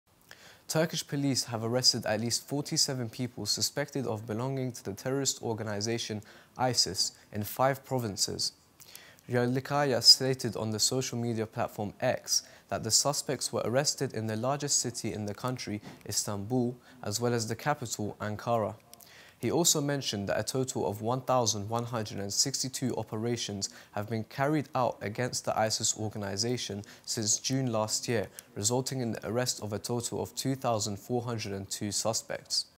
Turkey police arrest at least 47 ISIS suspects across country
Turkish police conducted raids in five provinces, resulting in the arrest of 47 individuals suspected of ties to ISIS. Interior Minister Yerlikaya confirmed arrests in Istanbul and Ankara, part of 1162 operations since June, totaling 2402 arrests.

Turkish police have arrested at least 47 people suspected of belonging to the terrorist organization "ISIS" in five provinces.
Yerlikaya stated on the social media platform "X" that the suspects were arrested in the largest city in the country, Istanbul, as well as the capital Ankara.
He also mentioned that a total of 1162 operations have been carried out against the ISIS organization since June last year, resulting in the arrest of a total of 2402 suspects.